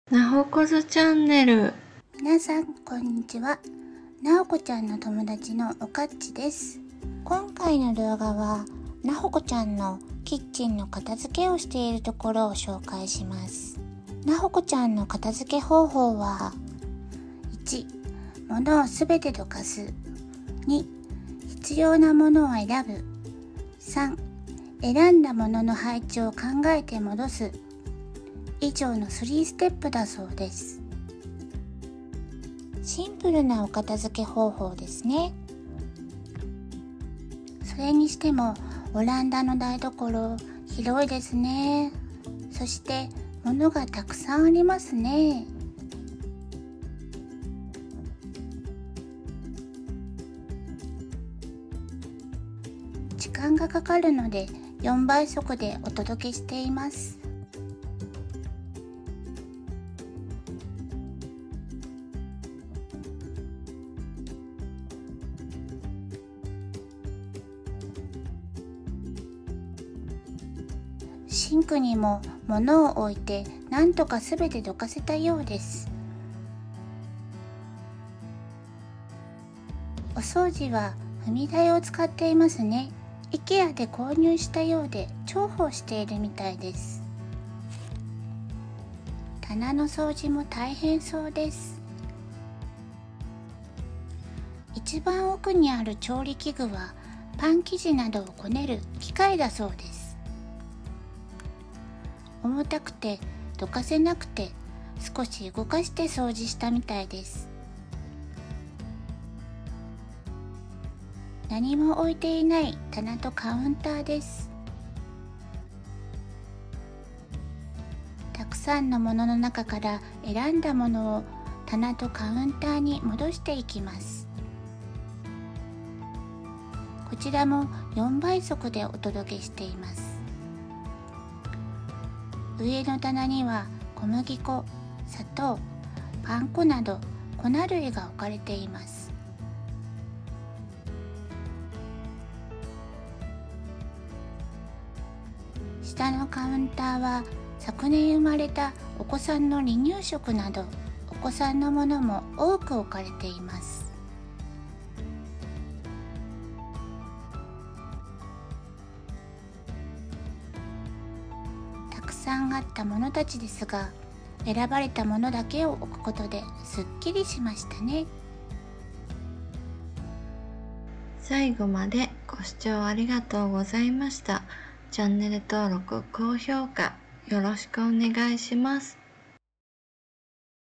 【オランダ生活】片付け｜キッチンの棚とカウンター｜3ステップ｜シンプル｜ときめくものを残す【Vlog】
こんにちは！
オランダに住むなほこです。今回の動画は我が家のキッチンのカ棚とカウンターの片付けを紹介します。
主な内容
・キッチンの棚とカウンターの物を全てどかす
・片付け手順の紹介
・選んだものを棚とカウンターに戻す

【補足】
片付け終えた様子で包丁セットが入っていませんでした💦
包丁セットは今でも使用しています！

今回友達のオカッチちゃんにナレーションをお願いしました！
オカッチちゃんのナレーションをぜひお楽しみください✨

関連動画
【オランダ生活】片付け｜コート掛けと棚｜シンプル｜見直し｜ときめくものを残す【Vlog】
【オランダ生活】息子と3人初めてのお出かけ｜マーケット｜チョコレート屋さん｜購入品紹介【ミドルブルグ】